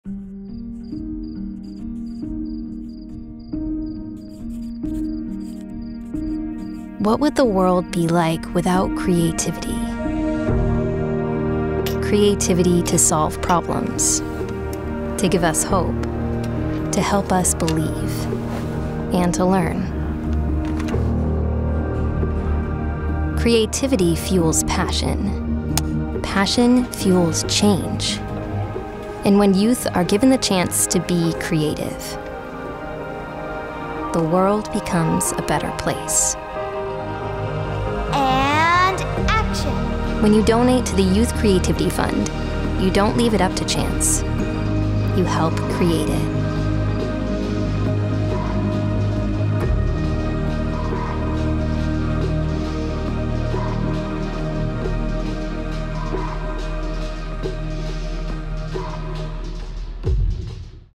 Don’t Leave it up to Chance
In support of the Youth Creativity Fund established to inspire youth across Waterloo Region to apply their creativity to solving problems. To learn more or make a gift to help build up the fund, go to: youthcreativityfund.ca .

What would the world be like without creativity?

To solve problems.

To give us hope.

To help us believe

And to learn…

Creativity fuels passion

Passion fuels change.

And when youth are given a chance to be creative
The world becomes a better place.

leave it up to chance.
You help create it.